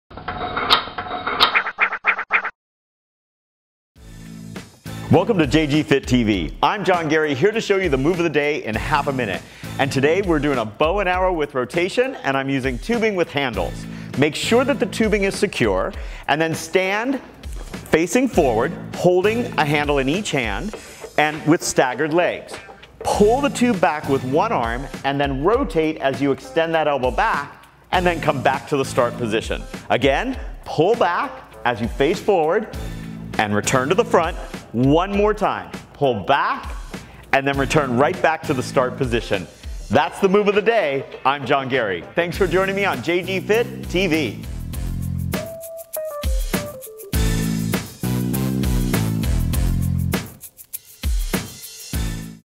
Bow and Arrow
Today's Move of the Day is Bow & Arrow using Tubing.  This is a great complex movement pattern.  It primarily focuses on the posterior chain. Change the attachment level to vary the muscular emphasis.